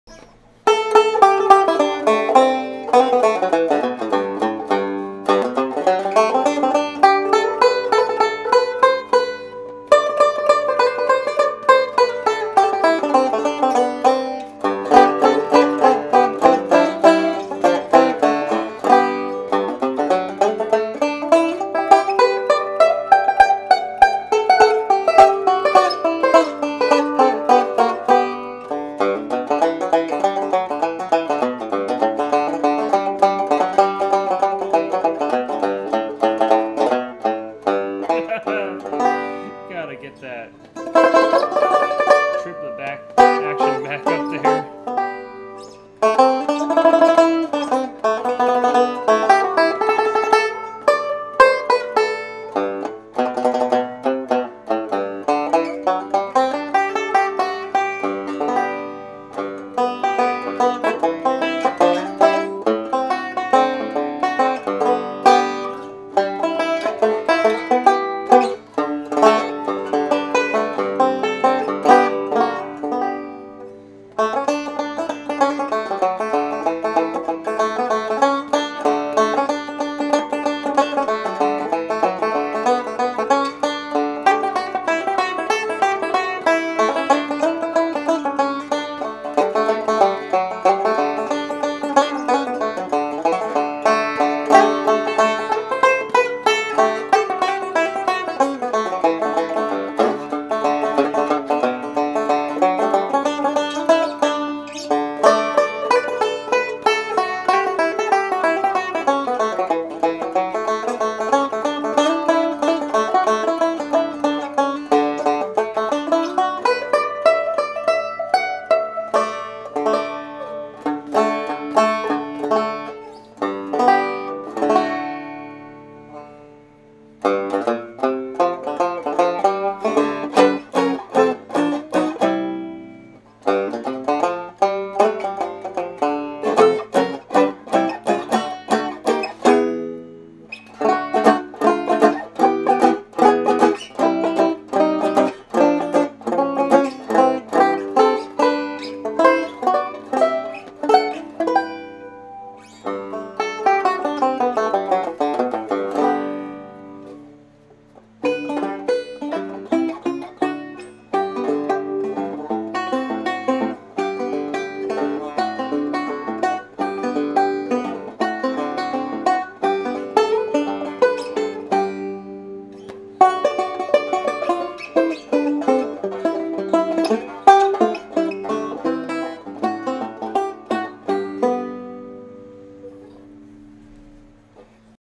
1922 Vega Style M Tubaphone tenor banjo
This is a customer’s 1922 Vega Style M Tubaphone tenor banjo. He has it tuned “Celtic” and it has a huge 11 3/4” rim. Of course it sounds good!

My work was just to reset the dowel and set it up. It’s all roses now…